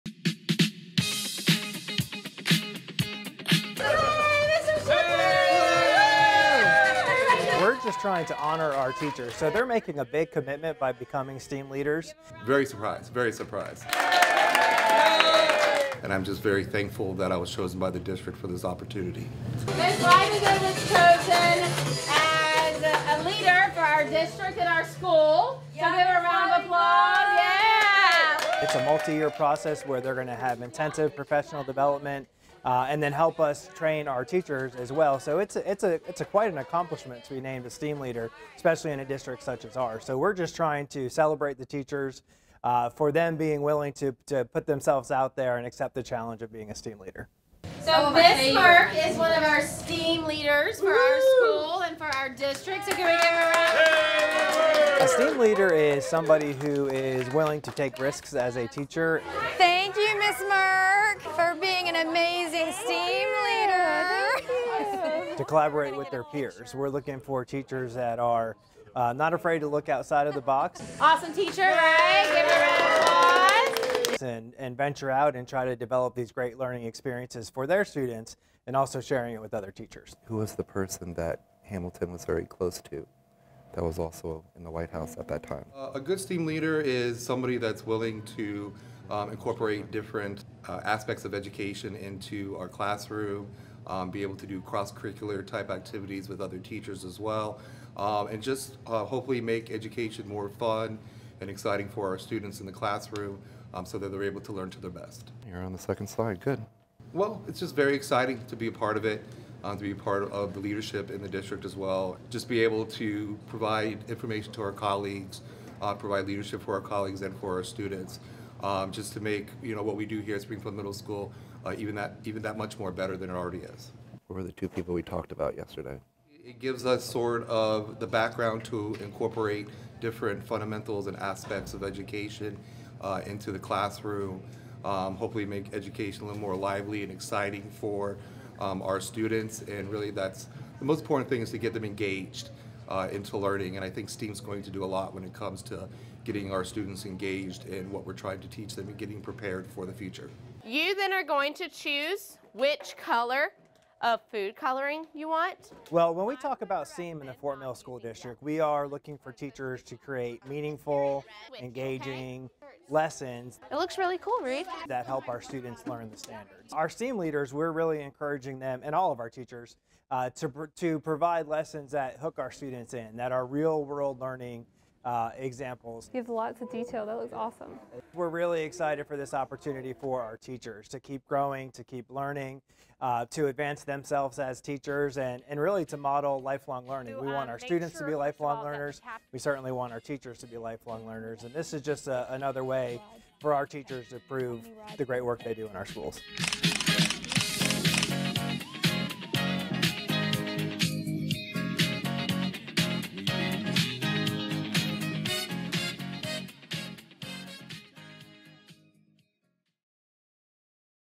STEAM AWARDS Springfield Middle School
The Fort Mill School District celebrated teachers being honored as new STEAM leaders. One of the district's STEAM leaders, Springfield Middle School eighth grade teacher Garrett Shipley, feels that STEAM will help his students to stay engaged with the curriculum and prepare them for the future.